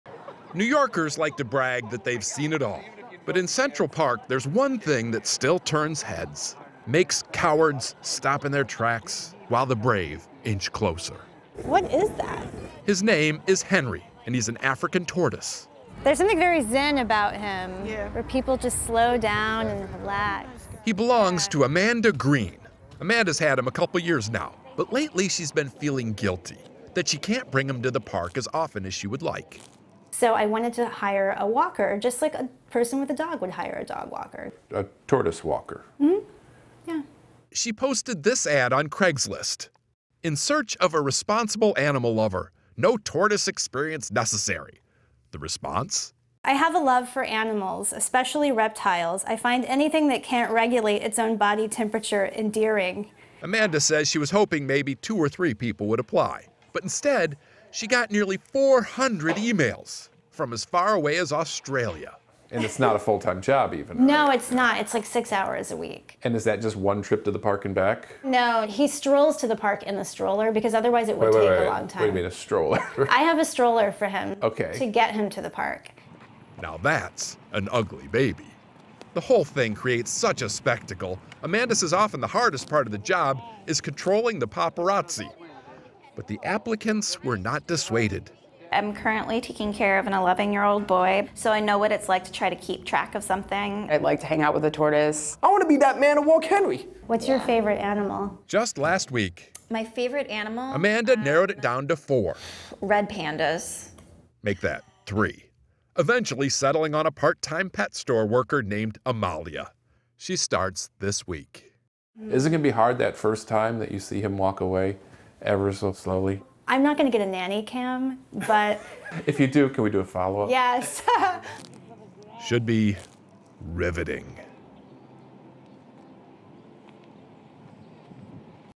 Seeking: NYC tortoise walker
Amanda Green doesn't have enough time to walk her pet, and she feels guilty because it's not getting the exercise it needs. So she's looking for a walker. The only problem is it takes a bit longer to walk her pet than most. Steve Hartman reports on an unusual help wanted ad, and the response it received.